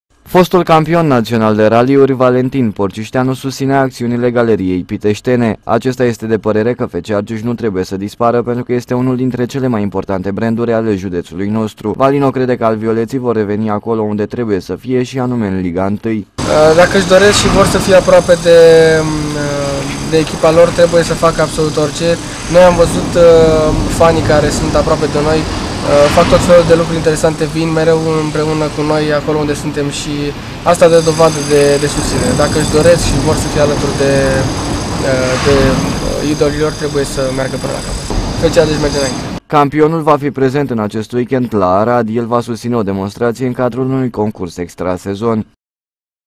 fostul campion national de raliuri Valentin Porcisteanu sustine aceasta actiune
Suporterii celor de la FC Arges vor organiza in aceasta seara de la ora 18 30 un mars de sustinere al echipei. Desi nu va fi in Pitesti fostul campion national de raliuri Valentin Porcisteanu sustine aceasta actiune si crede ca alb violetii trebuie sa revina in Liga I.